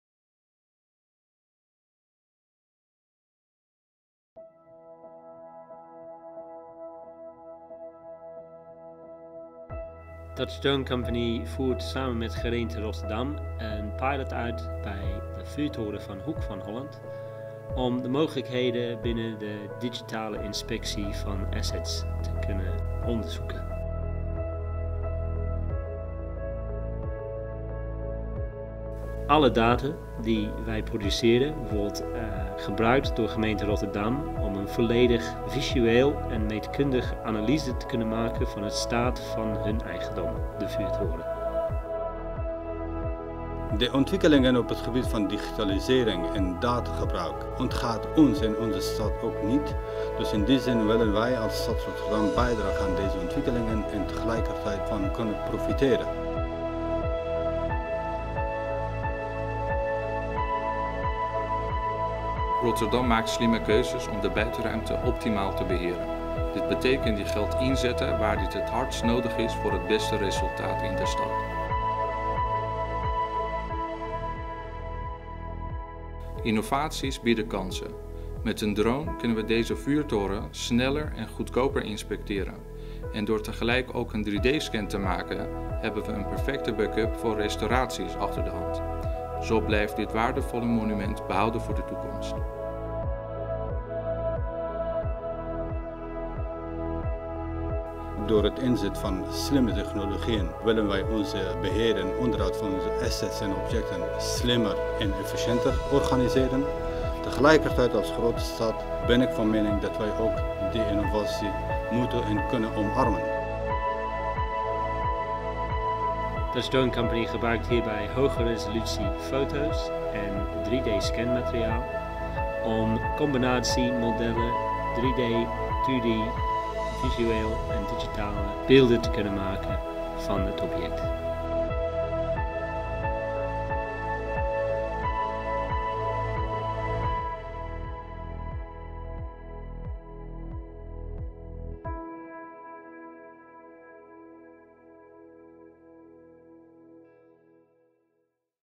Rotterdam vuurtoren
Hoe kunnen we Innovatie gebruiken voor maatschappelijke uitdagingen? Die vraag staat centraal tijdens de Innovation Expo 2018 in Rotterdam. Een goed voorbeeld hiervan is het gebruik van drones voor kwaliteitsinspectie van gebouwen, bruggen en tunnels.